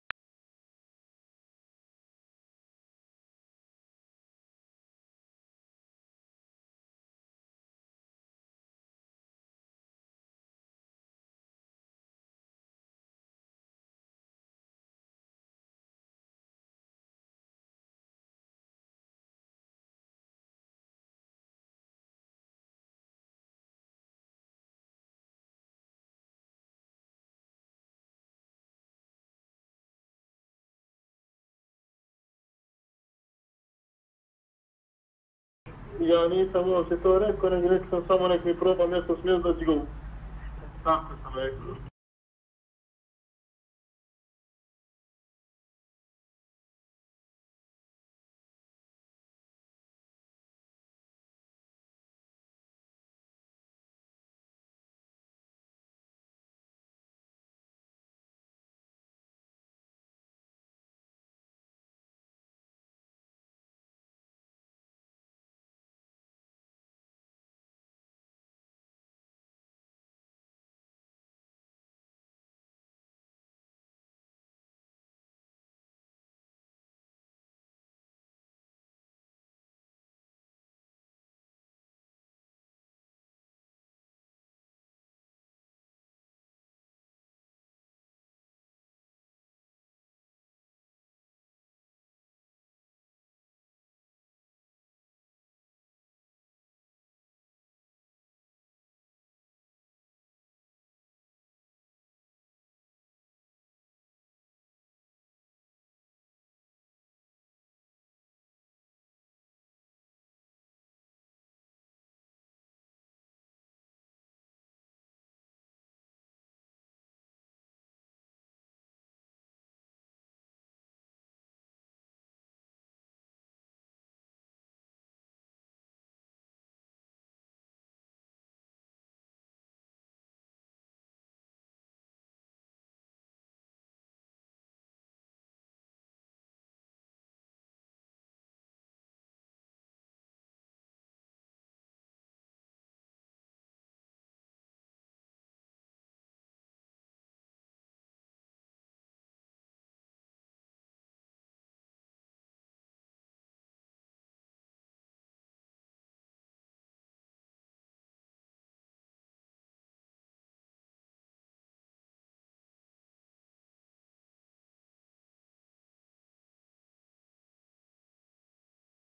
Miro Matijević: Vrh Hrvatske mafije su Mladen Bajić i Ranko Ostojić
Nakon višegodišnjeg rada i višegodišnjeg progona od strane Hrvatske mafije, preko Like sam dokazao da su vođe te mafije Mladen Bajić i Ranko Ostojić. Posjedujem sve dokaze i ovim putem sve prijavljujem Državnom odvjetniku DINKU CVITANU.
Uz to, postoji opravdana sumnja da je Mladen Bajić preko supruge   Ecije Bajić-Kuljiš kroz privatizaciju uništio Splitsko gospodarstvo, kao i za sramne radnje u dogovoru sa psihijatrom Goranom Dodigom.